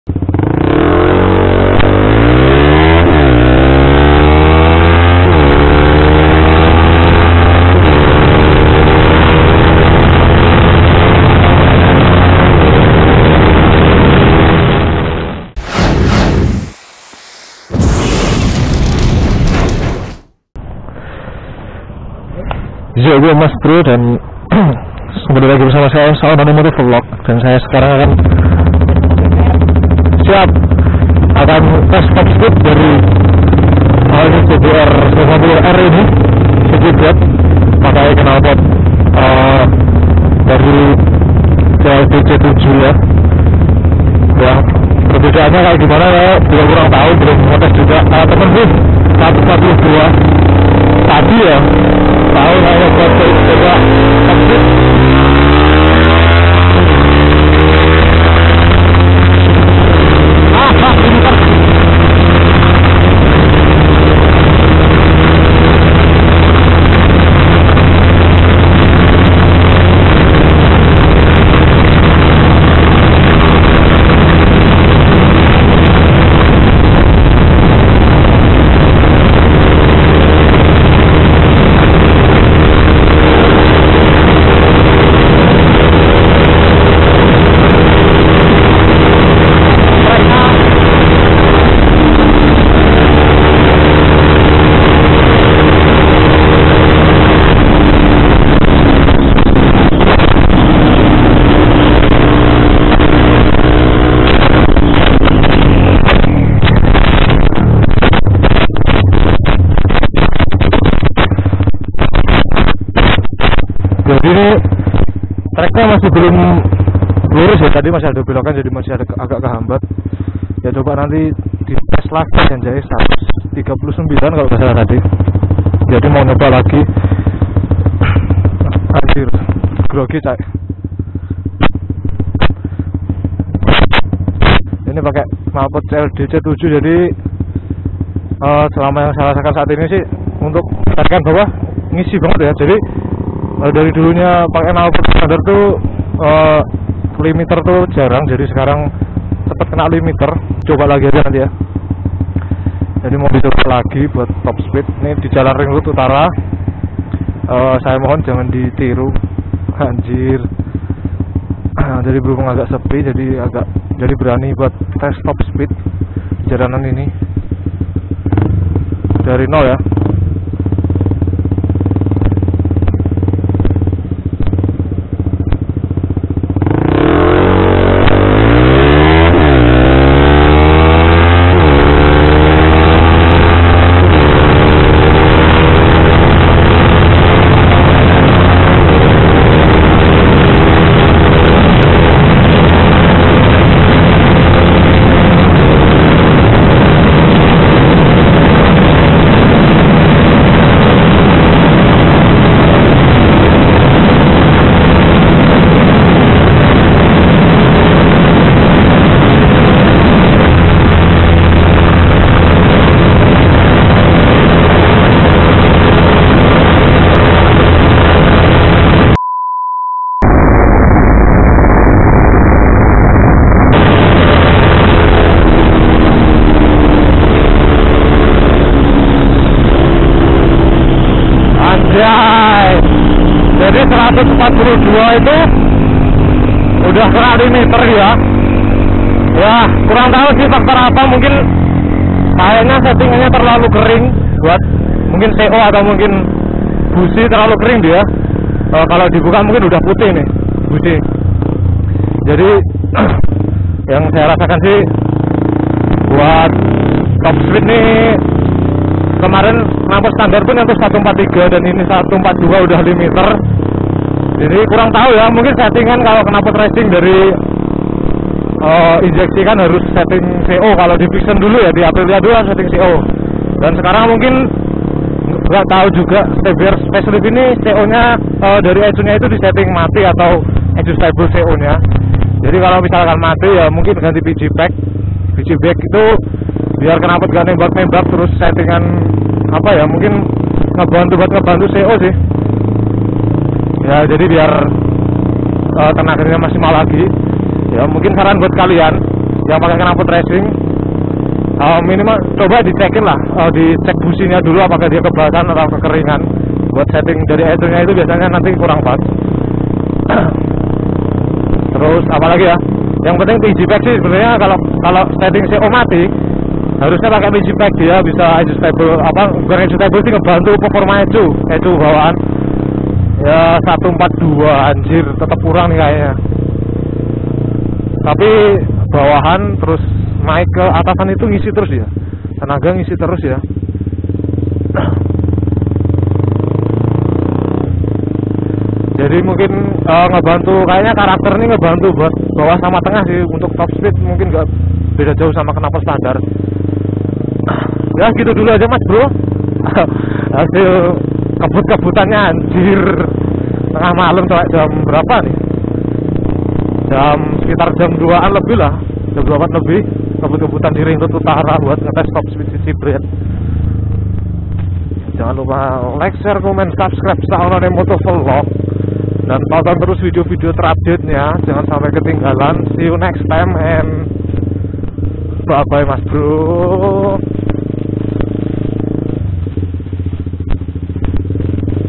TOP SPEED CBR 150R FACELIFT KNALPOT CLD C7
Sebelum menonton video ini saya kembali berpesan agar tidak meniru hal seperti ini. Saya akui saya nggatheli dan tidak tertib lalu lintas.

Action Camera : BCam X-3W Mic External ( Set 2.7K Editing jadi 720p )
Helm : KYT R-10 Red Fluo
Knalpot : Silencer CLD C7 Oval feat header HOGO